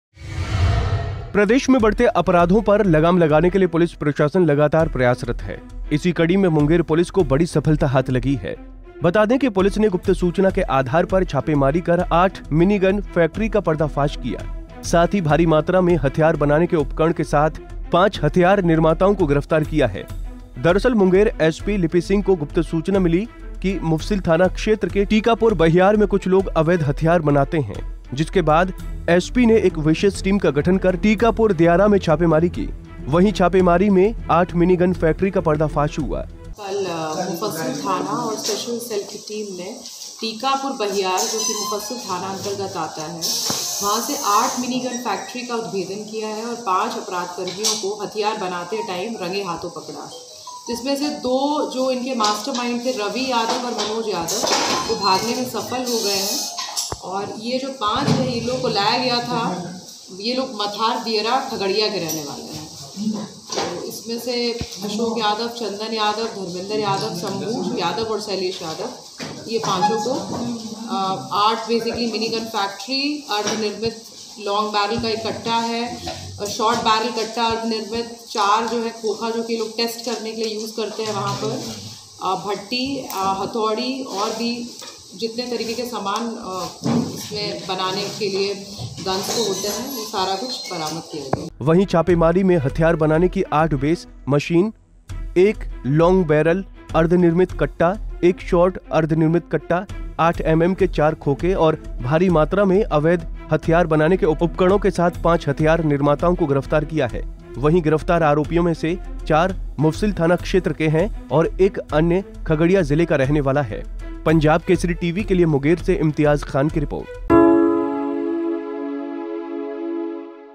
Munger पुलिस ने छापेमारी कर पकडी मिनी GUN फैक्ट्री, पांच लोग गिरफ्तार
Munger MungerPolice Minigunfactory # .प्रदेश में बढ़ते अपराधों पर लगाम लगाने के लिए पुलिस प्रशासन लगातार प्रयासरत है. इसी कड़ी में मुंगेर पुलिस को बड़ी सफलता हाथ लगी है. बता दें कि पुलिस ने गुप्त सूचना के अधार पर छापेमारी कर 8 मिनी गन फैक्टरी का किया पर्दाफास किया है. साथ ही भारी मात्रा में हथियार बनाने के उपकरणों के साथ 5 हथियार निर्माताओं को किया गिरफ्तार किया है..